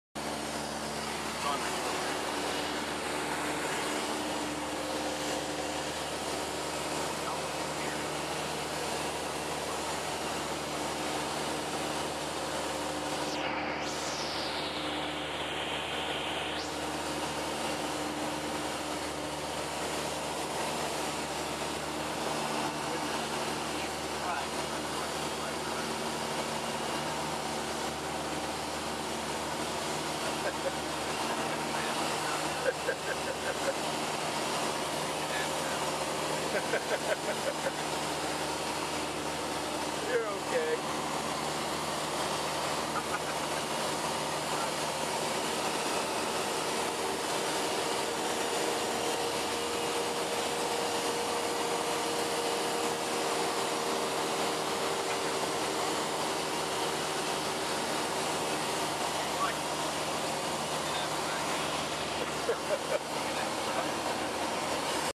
Jeremy Doing a Fine Job Flying
Flew down to Bloomington, IN for lunch (KBMG).  Cook Aviation is the BEST!  They have extremely nice courtesy cars, including Cadillacs and nice Buick.   You have to check them out if landing at Bloomington.